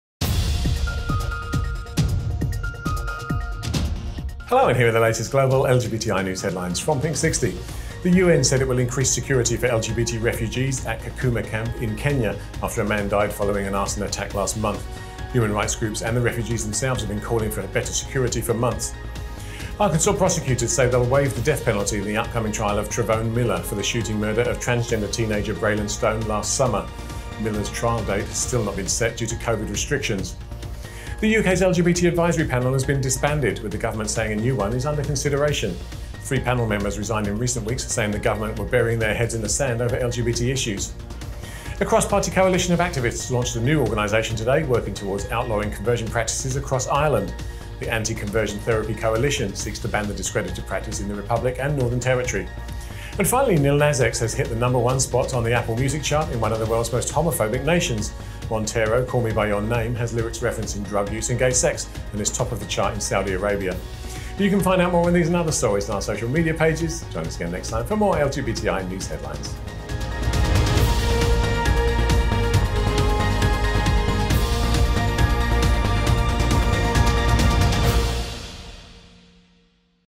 DEATH PENALTY WAIVED IN TRANS MURDER
PINKSIXTY NEWS I TUESDAY 13 APRIL 2021

Security increased after refugee killed
UK’s LGBT Advisory Board disbanded
Coalition aims to ban conversion practices
Lil Nas X tops music chart in Saudi Arabia

Produced by Pinksixty for OUTtvGo.com